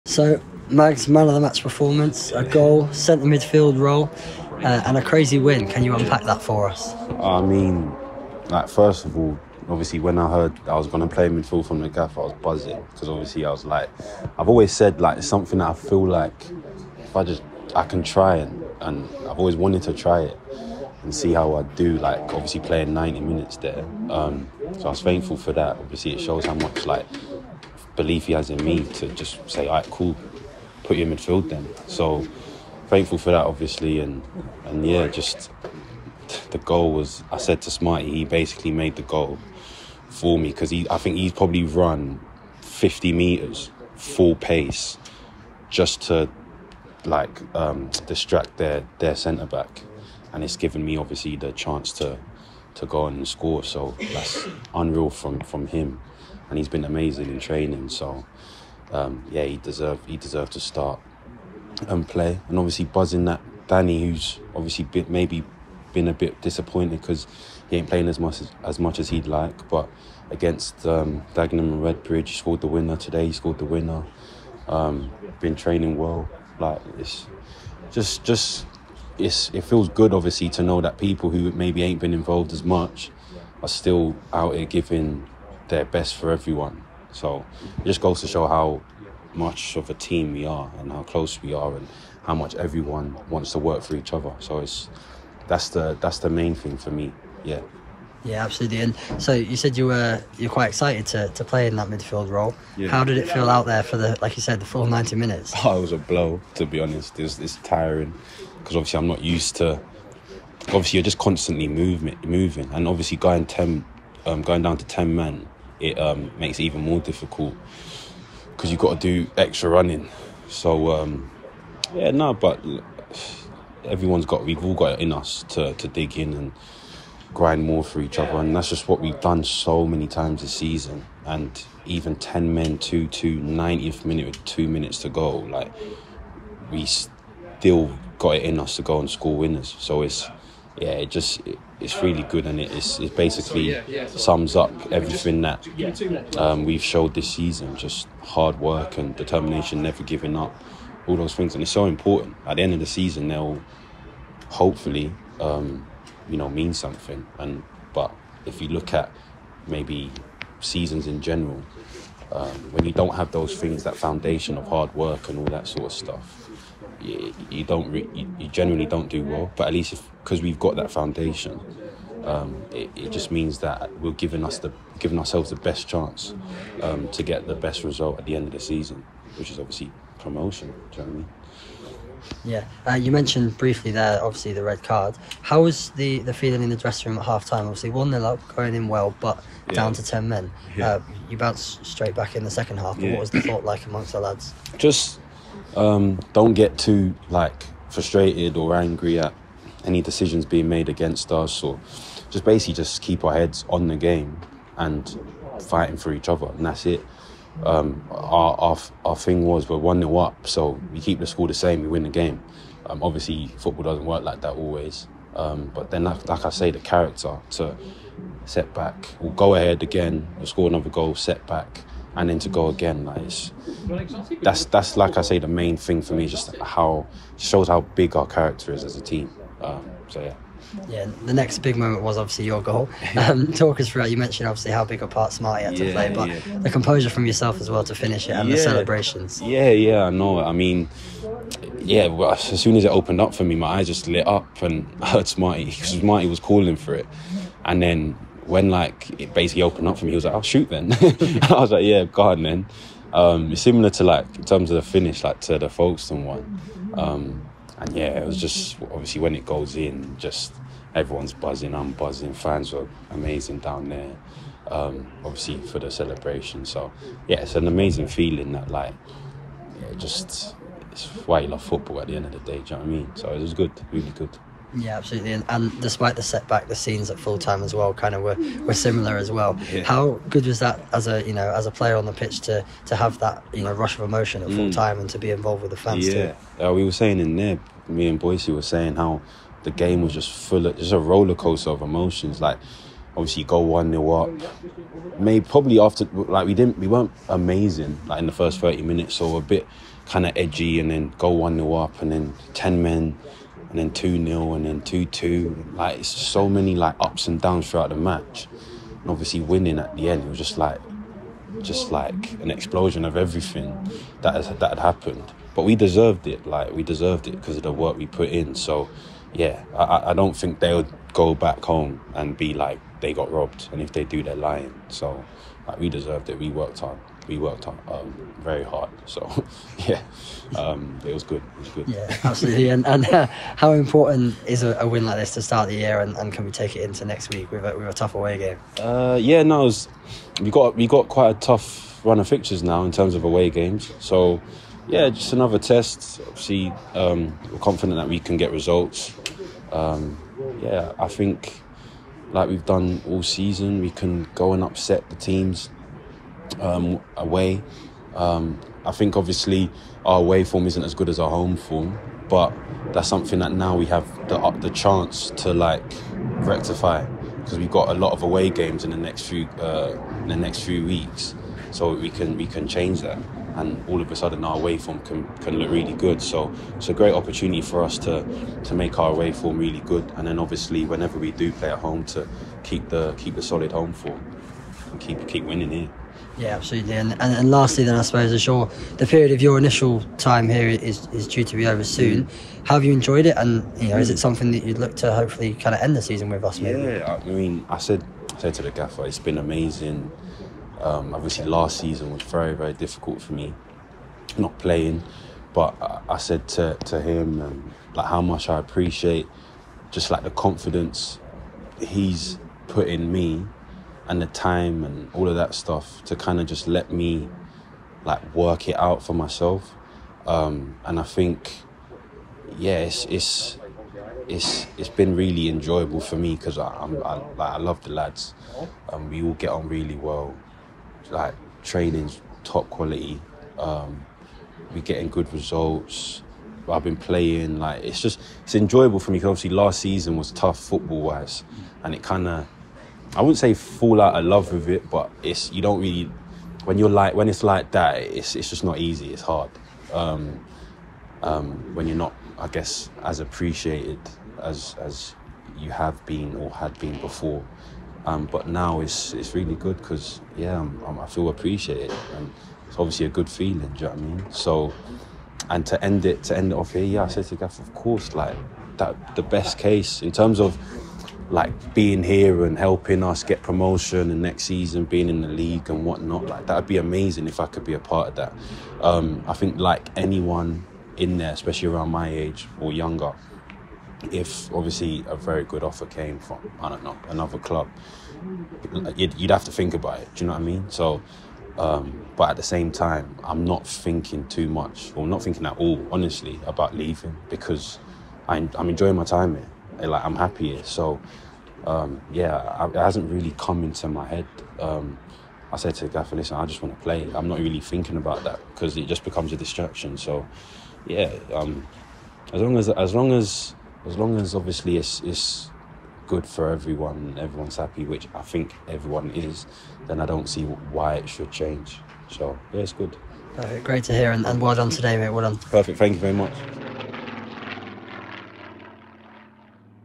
INTERVIEW | Christian Maghoma post-Weymouth Win
Eastleigh's Christian Maghoma shares his thoughts after his MOTM display in a midfield role against Weymouth.